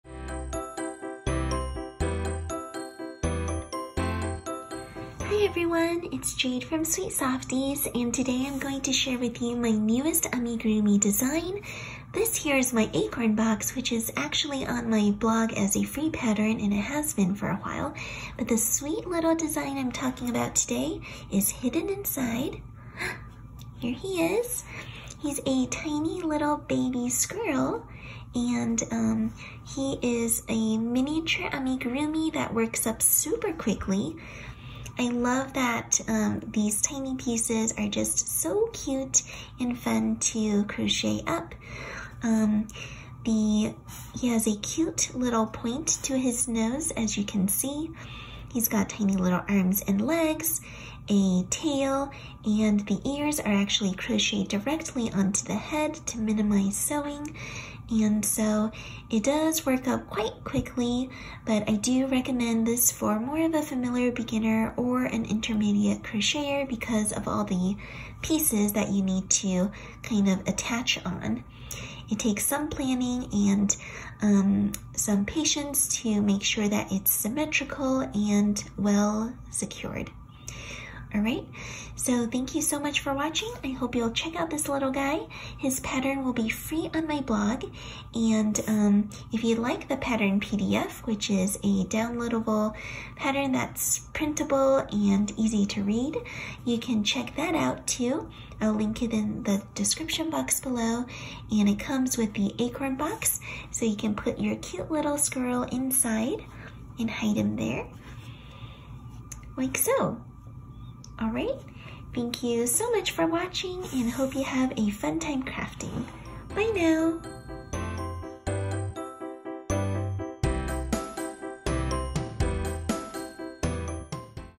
Baby Squirrel in Acorn Preview · Free Written Amigurumi Crochet Pattern
This is a preview of the free crochet pattern: Baby Squirrel in Acorn!  This mini amigurumi project is very quick to work up, and fits perfectly inside of my Acorn Box pattern.

About the Baby Squirrel Pattern
Size:
Height: 2.5 inches (6 cm)

Level of Difficulty:
★★★☆☆ Intermediate, due to having to sew some small pieces. Requires familiarity with stitches used (below), attaching yarn, attaching safety eyes, and sewing parts together.

Stitches/Terms (US Terms):
FO = fasten off
MR = magic ring
ch = chain
sl st = slip stitch
inc = increase
dec = decrease
Surface slip stitch

Materials:
• YARN ~ LBY Heartland
• Clover Amor Hook (2.75mm)